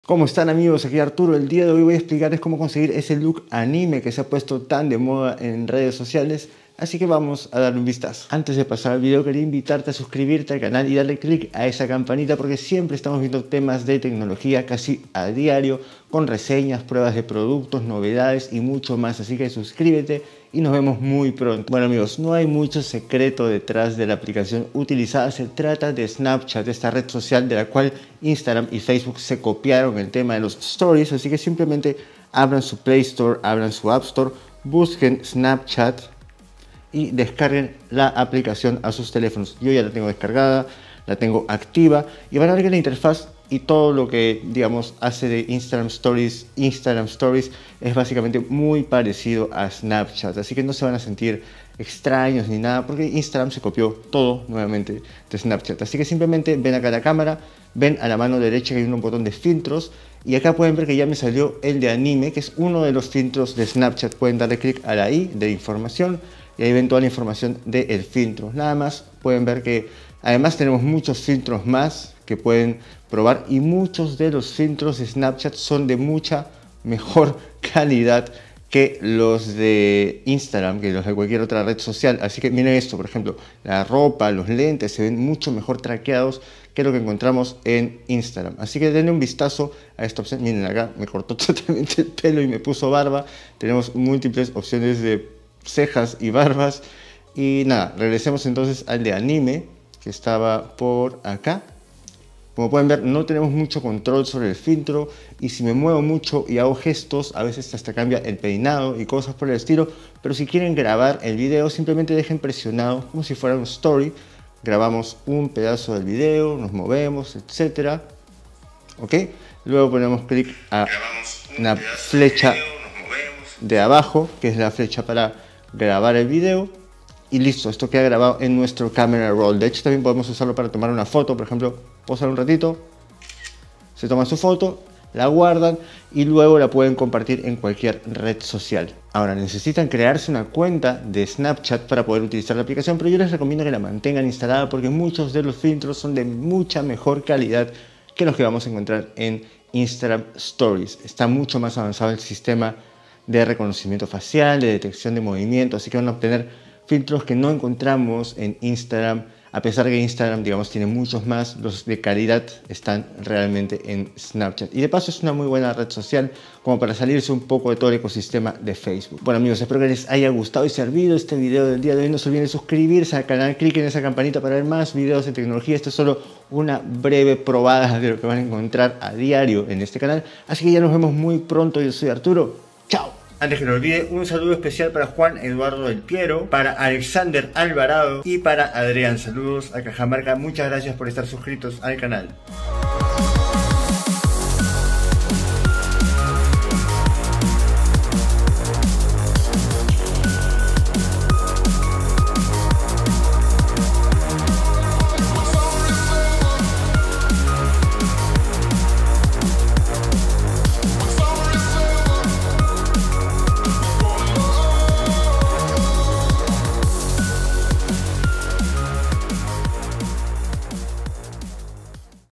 Filtro Anime para Instagram, TikTok, Snapchat. Cómo conseguirlo?
Te gustó el contenido? APOYA al canal con un YAPE

Quieres el filtro de Anime que se ha puesto taaan de moda? Acá te explicomo como obtener el look anime para tiktok, instagram o snapchat! Súper fácil!